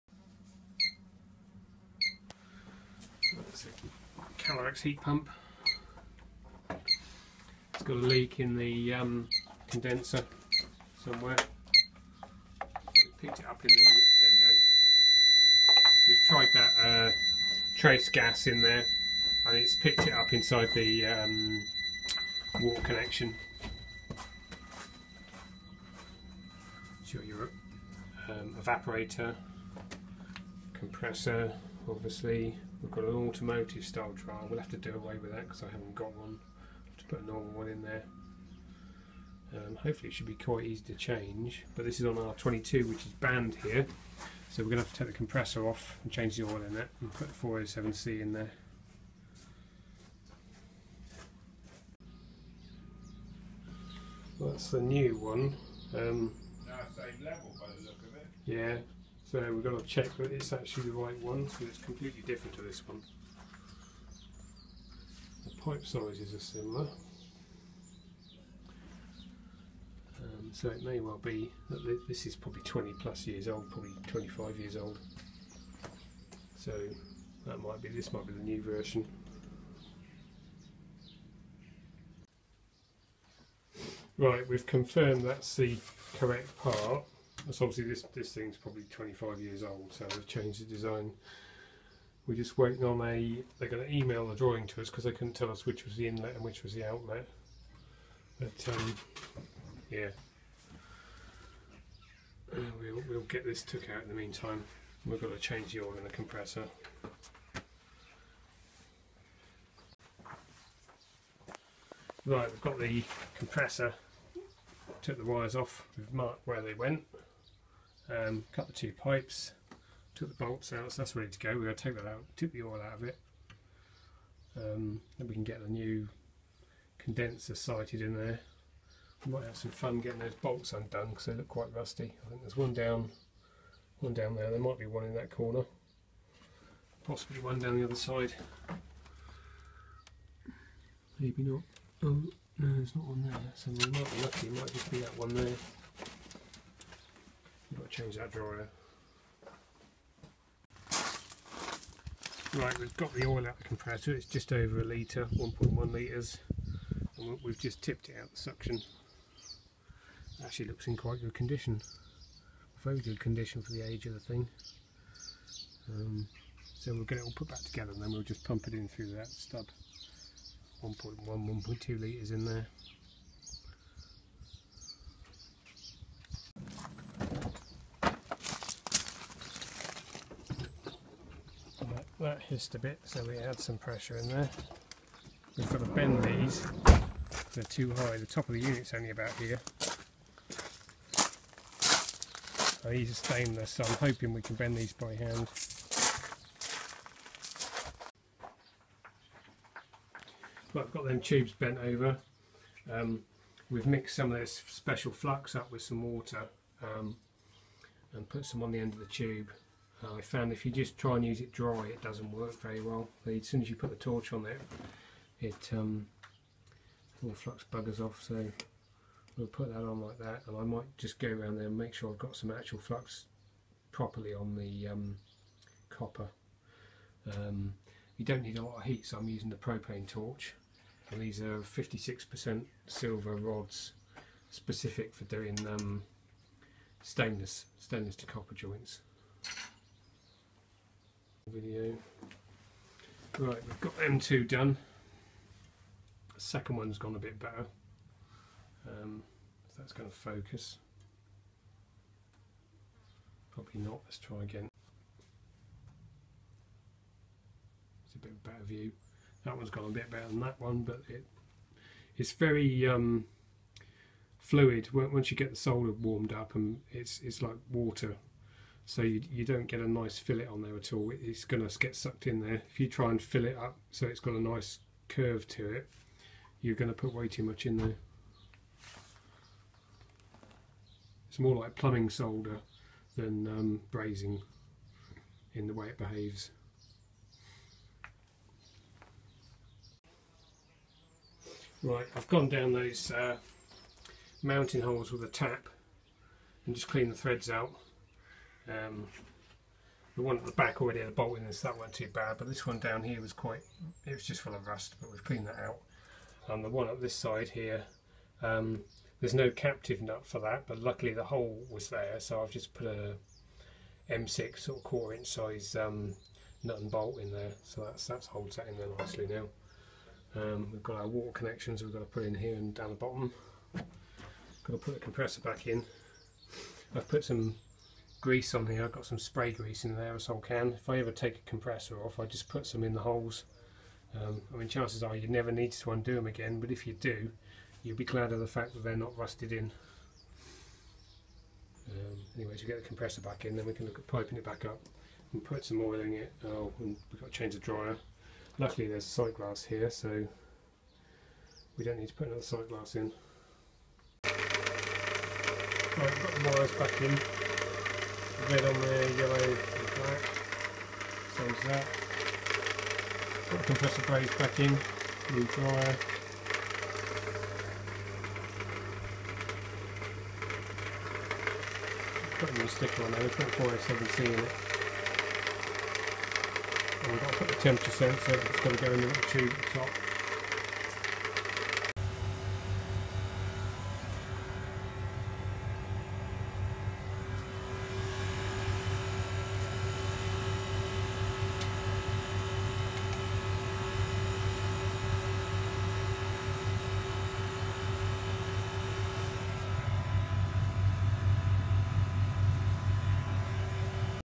Calorex Condenser Replacement
Found the system empty of refrigerant at the start of the swimming pool season, could not find the leak originally so used Trace-A-Gas and then found after leaving the system for a while that the condenser was the source of the leak.

Had some fun making the new style condenser fit in the older unit due to the different pipe connections.
We changed the oil in the compressor from mineral to synthetic and replaced the R22 with R407C